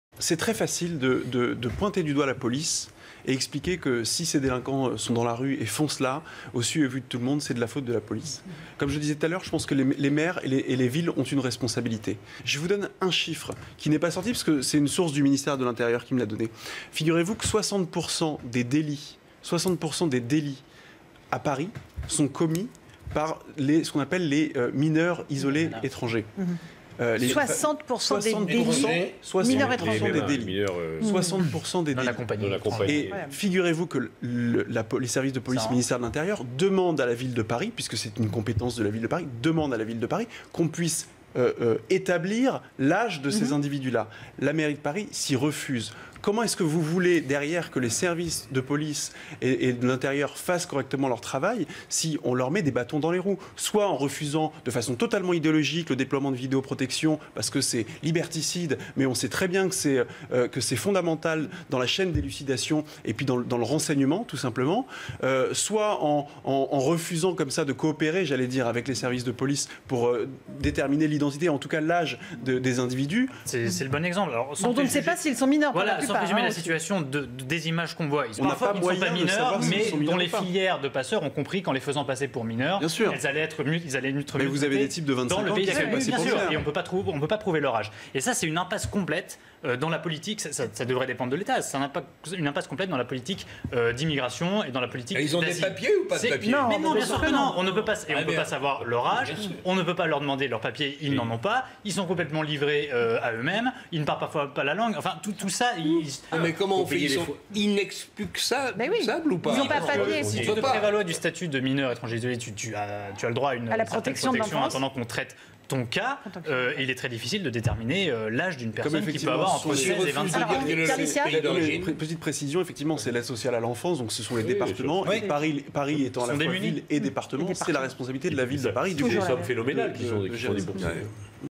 Lutte contre l'insécurité : quand la Ville de Paris fait obstruction
60% des délits à Paris sont commis par des mineurs étrangers non-accompagnés. Beaucoup d'entre eux sont vraisemblablement majeurs mais la Mairie de Paris refuse d'établir l'âge exact de ces individus malgré les demandes insistantes du ministère de l'Intérieur et de la Préfecture